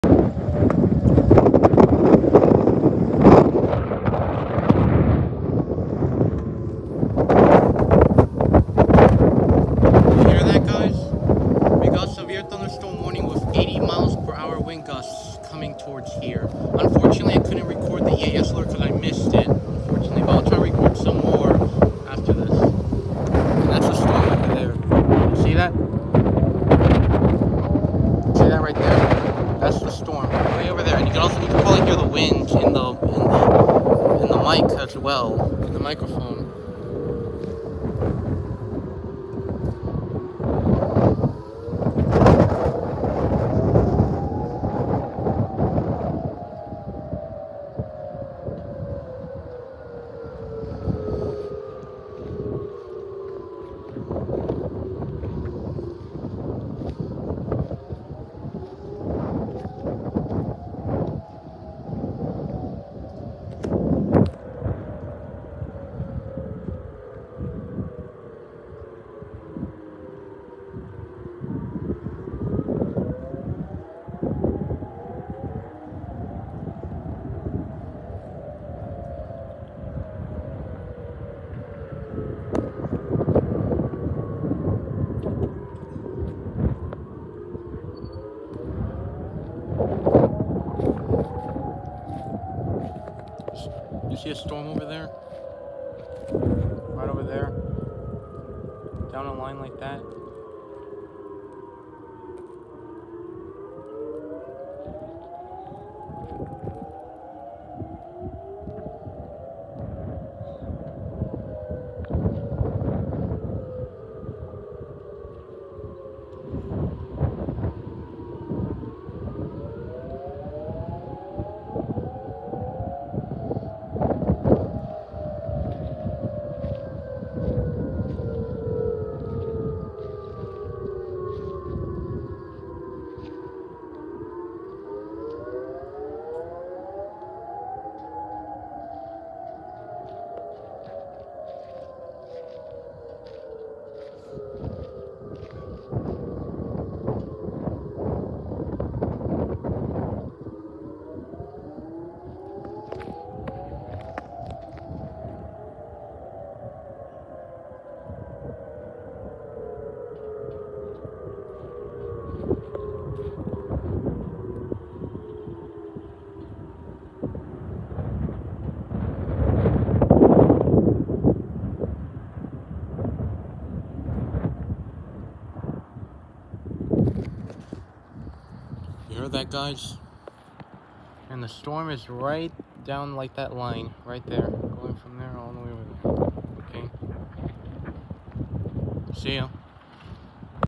Allen Texas Tornado Sirens #4 (NOT A TEST) (REAL ALERT) 3-2-23 at 6:18PM
This originally occurred on March 2, 2023 at 6:18PM.        Today there was a Particularly Dangerous Situation (PDS) Severe Thunderstorm Warning WITH 80 MILES PER HOUR WINDS that sounded the tornado sirens.      There was also a Particularly Dangerous Situation (PDS) Severe Thunderstorm Warning that happened on the weather alert radios, but unfortunately I missed the alert.     The sirens sounded 2 times today.          Unfortunately I couldn’t record the second activation because the storm with the strong winds had arrived, blowing everything around and knocking out the power so it wasn’t safe for me to walk outside anymore to film it because of that.      I apologize for all the wind noises that you might have heard in the video because of the strong winds with the storm coming into our area.